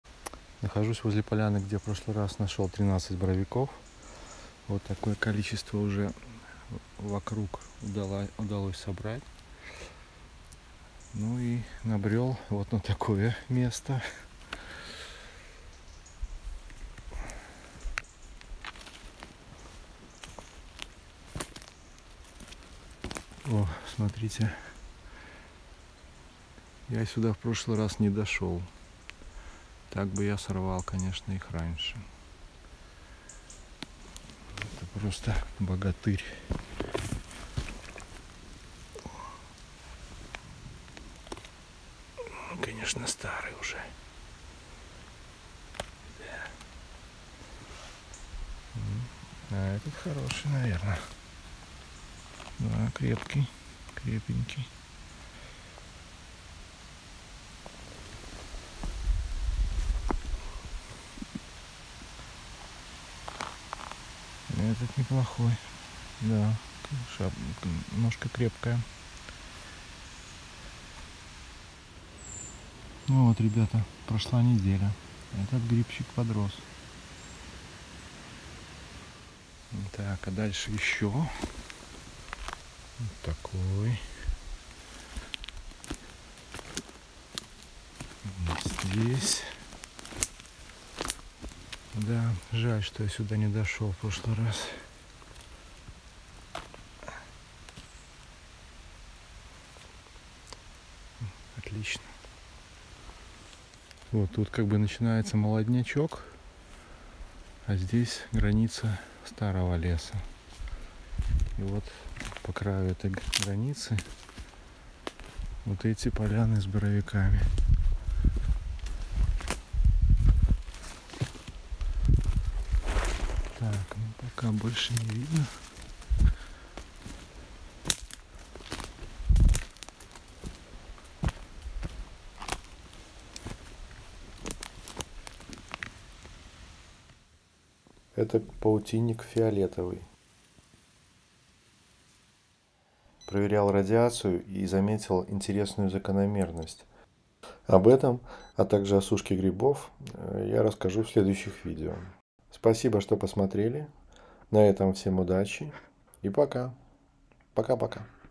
БОРОВИКИ ВЫРОСЛИ! The boletus have grown!
Снова приехал на место, где нашел поляну боровиков («ведьмин круг»). Оставленный маленький гриб вырос! Вокруг появились новые белые грибы. Обследовал всю местность вокруг, есть несколько новых полян с огромными боровиками. Места вокруг красивейшие, нашел лесную реку, но грибы были только на местах, удачно разведанных неделю назад. Нашел редчайший гриб - паутинник фиолетовый. Замеряю радиацию, придется продолжить это исследование, чтобы подтвердить или опровергнуть ряд предположений. Набрал ведро боровиков!

Again arrived at the place where I found a clearing of boletus ("witch's circle"). The little mushroom I had left had grown! New white mushrooms appeared around. Explored the whole area around, there are several new glades with huge boletuses. The places around are beautiful, I found a forest river, but the mushrooms were only in the places I had successfully explored a week ago. Found the rarest mushroom - Cortinarius violaceus (agaricus violaceus). Measuring radiation, will have to continue this research to confirm or refute a number of assumptions. Picked a bucket of boletus!